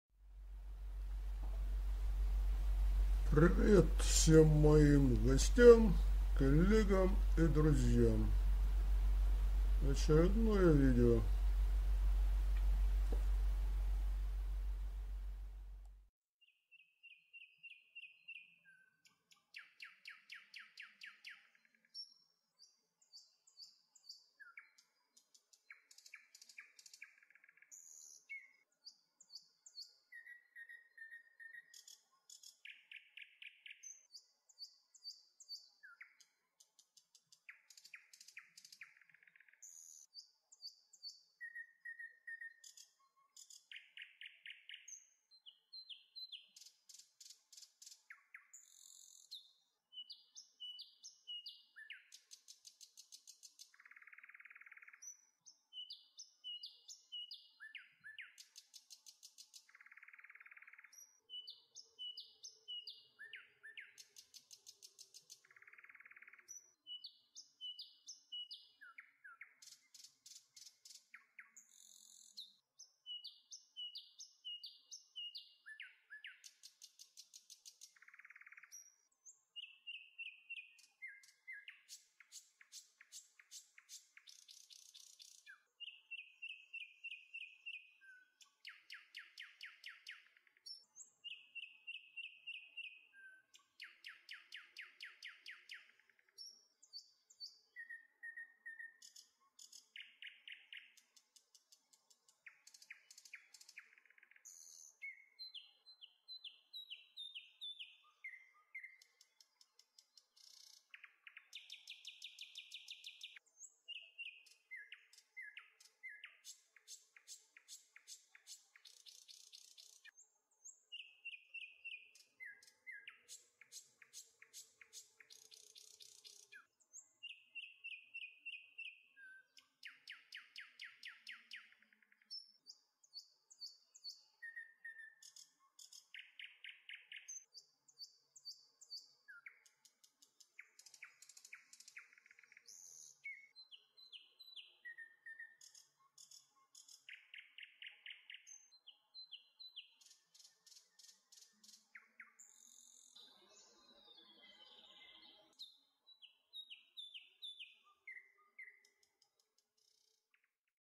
Aquarium. Лесная тропа. Archive.
В аквариумном лесу идёт тропинка в неведомою  даль.